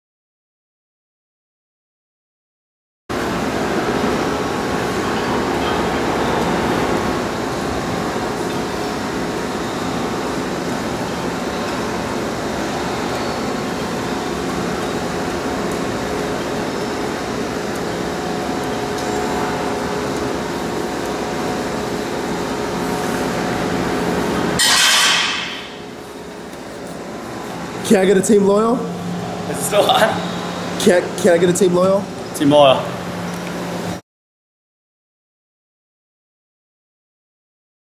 TeamLoyal - Twice The Speed - Jack Cascio
Team Loyal Jack Cascio is launching a new branch of the Twice The Speed Brand that is coming out within the next week. Goal setting.